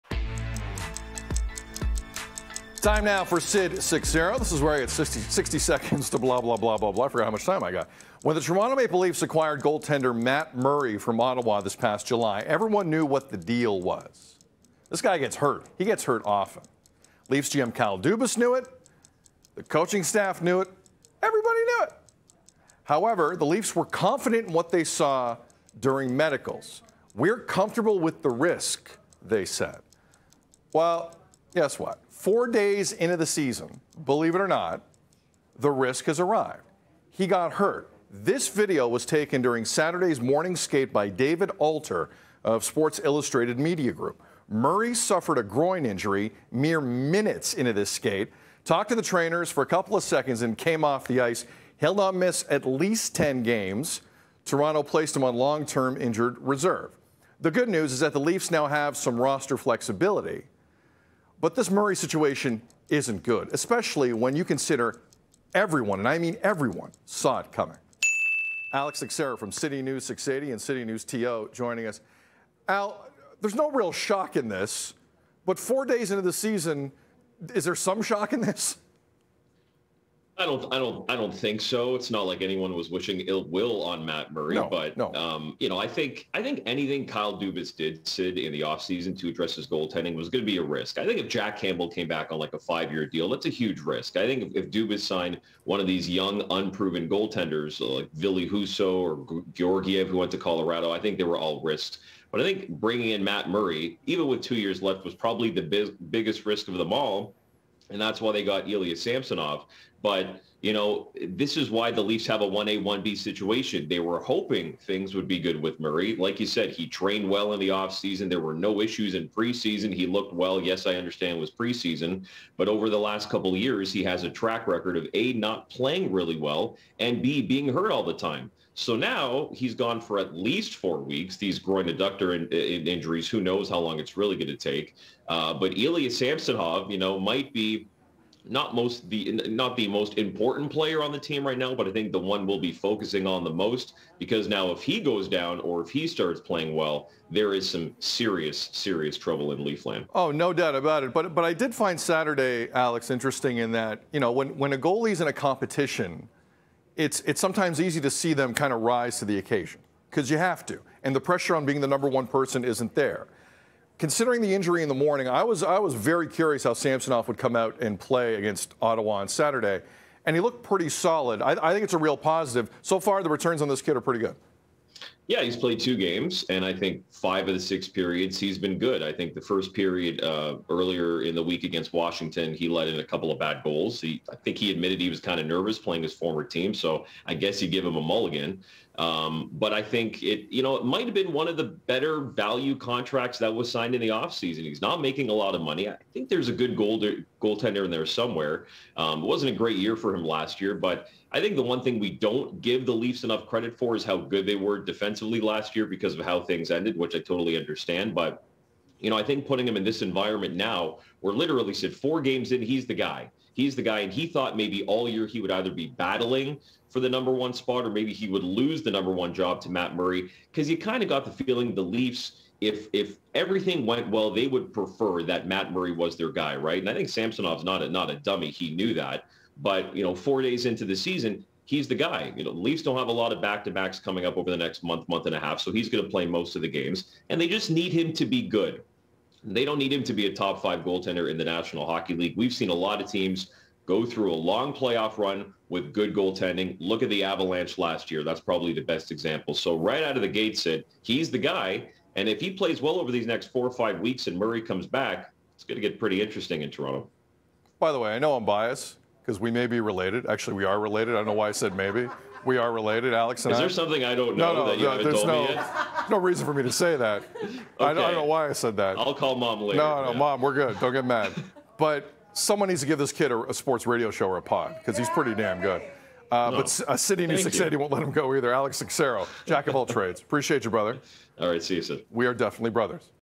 Maple Leafs' Matt Murray is out for at least a month due to injury
Sid is joined by Alex Seixeiro from CityNews to chat about Matt Murray's recent adductor injury.